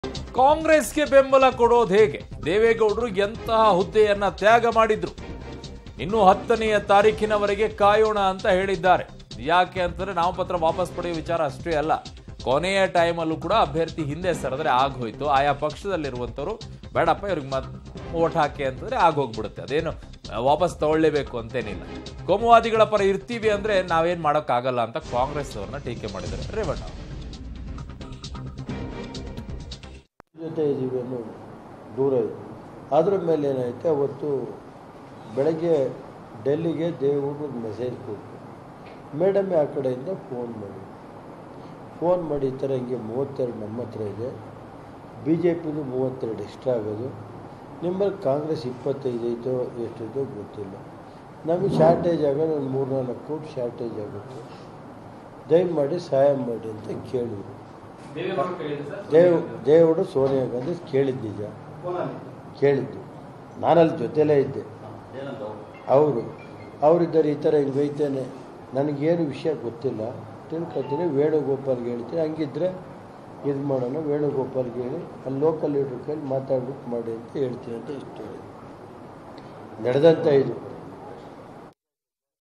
HD Revanna Seeks Congress Leaders Support | Rajya Sabha Election 2022 | Public TV
ಜೆಡಿಎಸ್ ನಾಯಕ ರೇವಣ್ಣ ಸುದ್ದಿಗೋಷ್ಠಿ.
ಕೋಮುವಾದಿಗಳನ್ನ ದೂರ ಇಡಬೇಕು.
ಜಾತ್ಯಾತೀತರು ಎಲ್ಲರೂ ಒಂದಾಗಬೇಕು.
ನಮ್ಮದು 32 ಓಟ್ ಇದೆ. 4-5 ಓಟ್ ಬೇಕು.
ಕಾಂಗ್ರೆಸ್ ಬಳಿ 25 ಓಟ್ ಇದೆ.
ನಮಗೆ ಬೇಕಾಗಿರೋದು ಕಡಿಮೆ ಓಟ್ ಮಾತ್ರ
ನಾಮಪತ್ರಕ್ಕೆ ಮುನ್ನ ಸೋನಿಯಾ ಗಾಂಧಿ ಜೊತೆ ದೇವೇಗೌಡರು ಚರ್ಚಿಸಿದ್ದಾರೆ